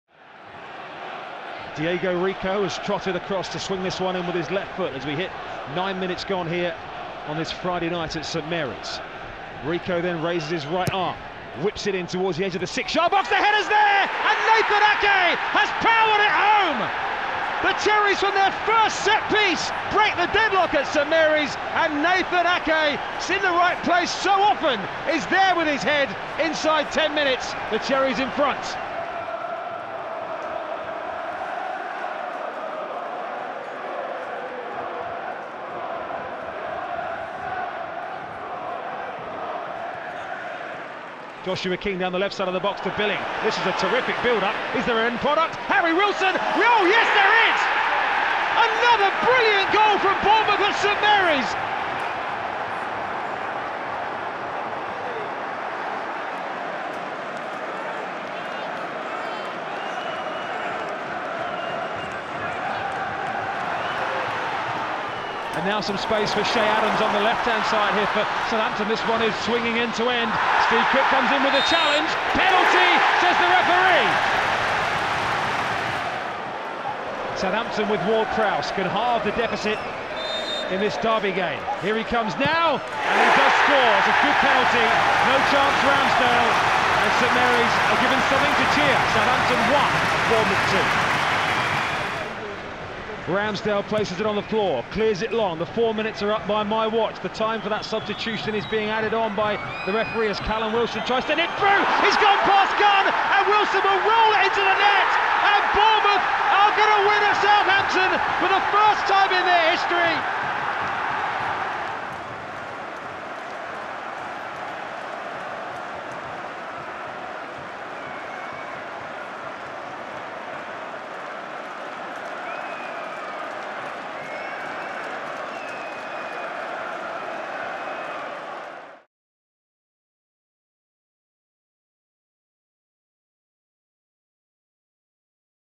Top performance in memorable derby win 💥| Southampton 1-3 AFC Bournemouth
See all the goals in a historic derby win as AFC Bournemouth beat Southampton 3-1 at Saint Mary's.